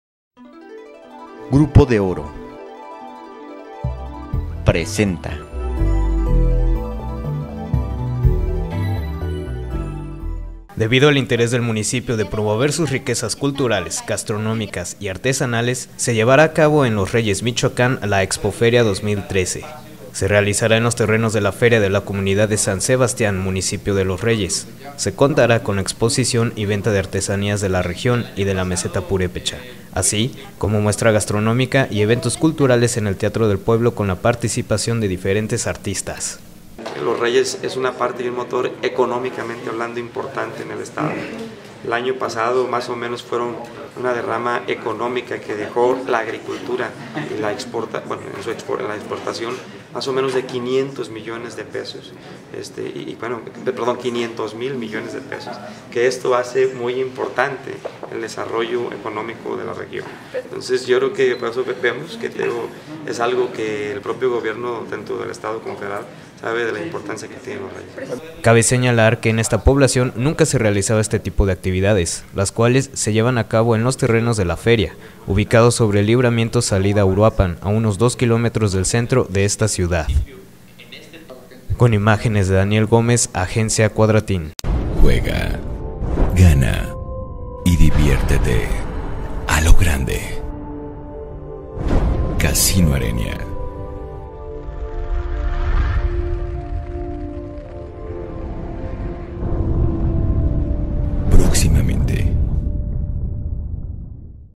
Los Reyes, importante motor económico en el estado: Antonio Salas
De acuerdo con comunicado de prensa se llevará a cabo en Los Reyes Michoacán La Expo Feria 2013, debido al interés del municipio de promover sus riquezas culturales gastronómicas y artesanales